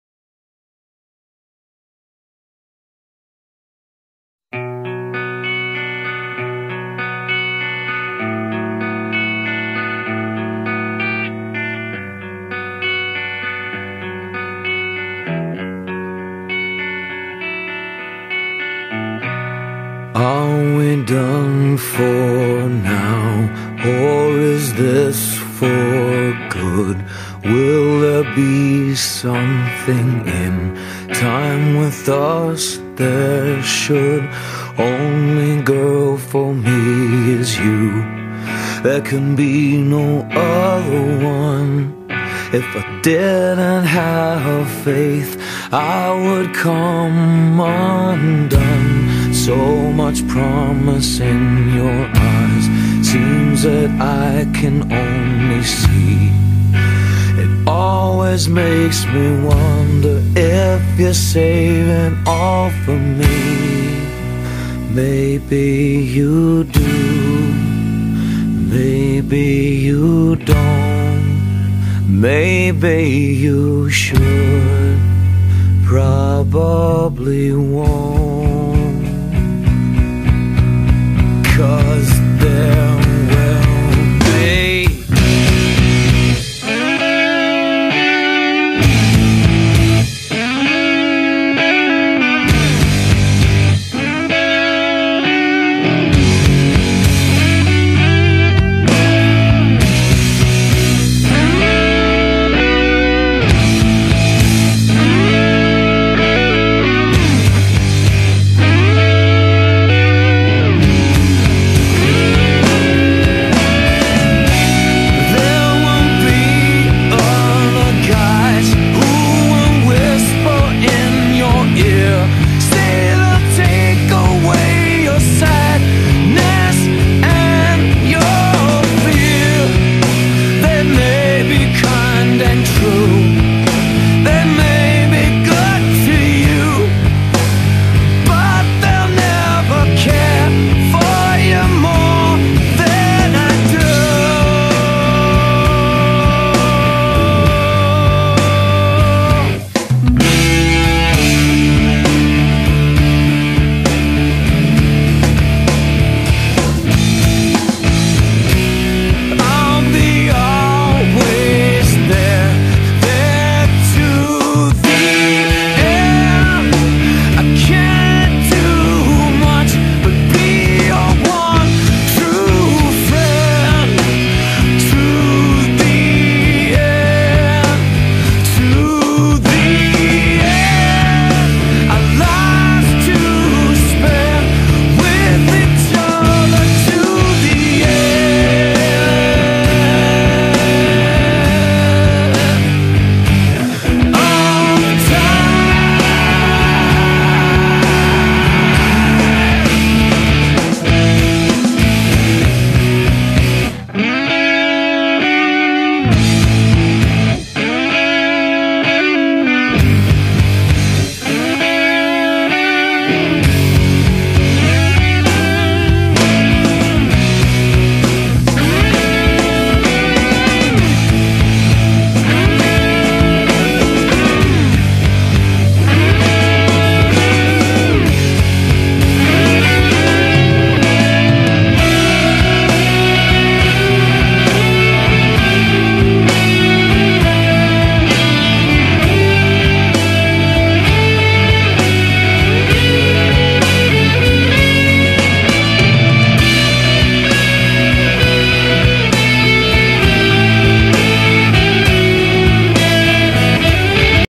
james marsters and ghost of the robot
this is a video with photos of james marsters and his band ghost of the robot